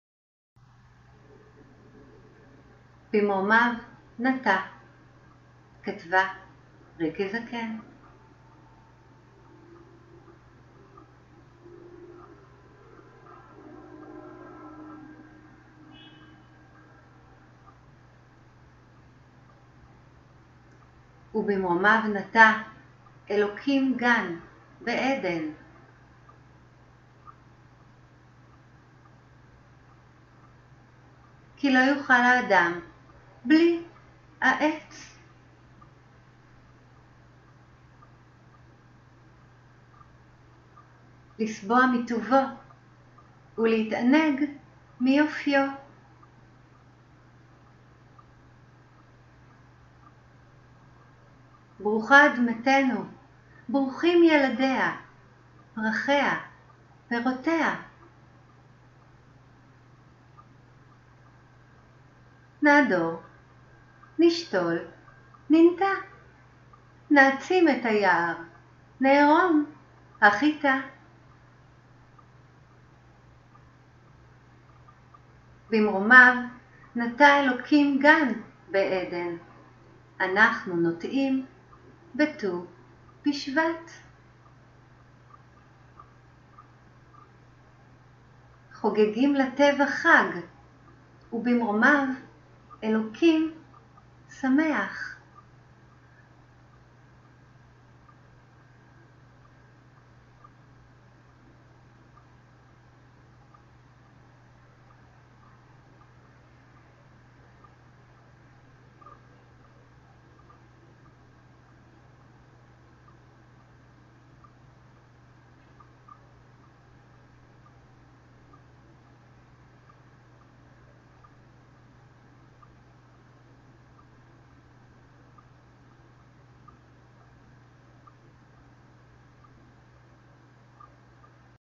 במרומיו נטע סיפור לטו בשבט כתבה: ריקי זקן
במרומיו נטע אלוקים גן בעדן, אנחנו נוטעים בטו בשבט
סיפור ילדים קצר לטו בשבט